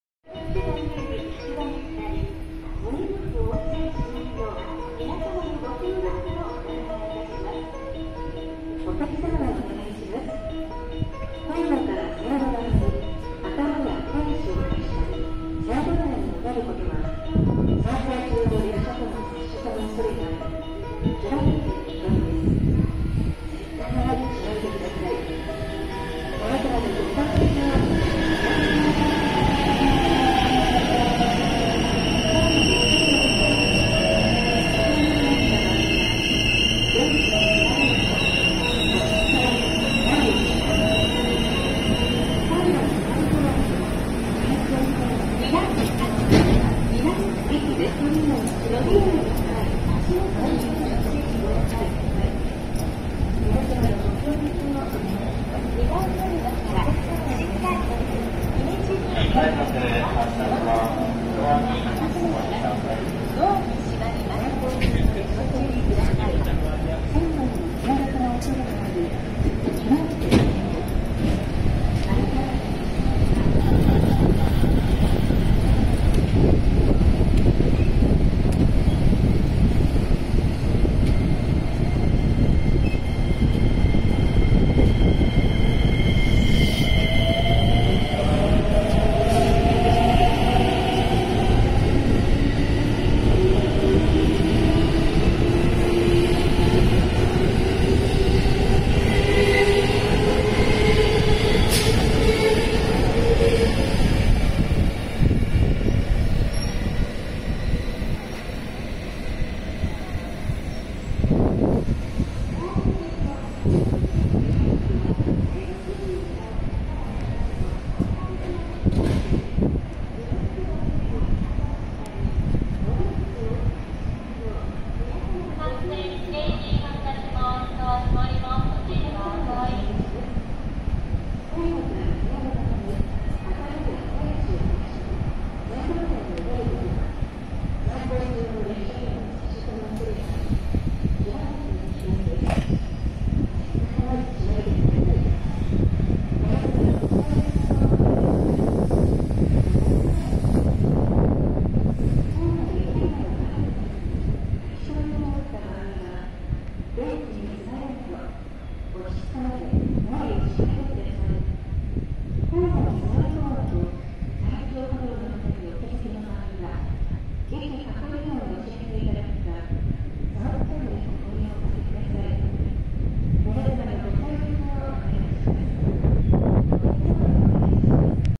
JR西日本 225系0番台 I編成+223系2000番台 V編成 新快速 播州赤穂行き 221系 K編成×2 草津線直通C普通 柘植行き 女性車掌さん 南草津駅 20240815
JR西日本

225系0番台 I編成+223系2000番台 V編成
新快速 播州赤穂行き+姫路行き

 221系 K編成×2
草津線直通C普通 柘植行き
女性車掌さん

南草津駅